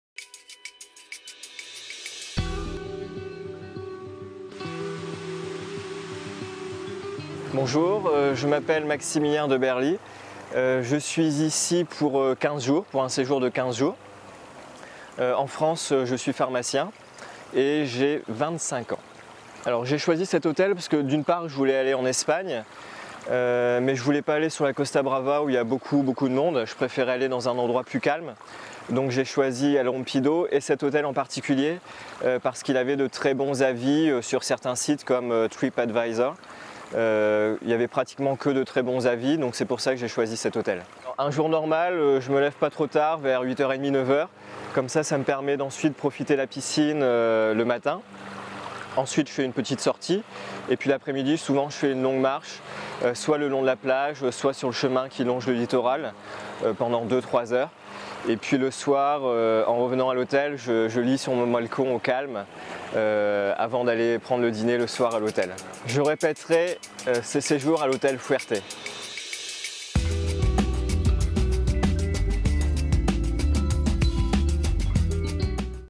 Fuerte El Rompido, Cartaya, Costa de la Luz, Huelva - Commentaires des clients - 10 fra
En savoir plus sur nous à travers les commentaires des clients qui ont choisi Fuerte El Rompido passer leurs vacances. Hôtel Cartaya, Costa de la Luz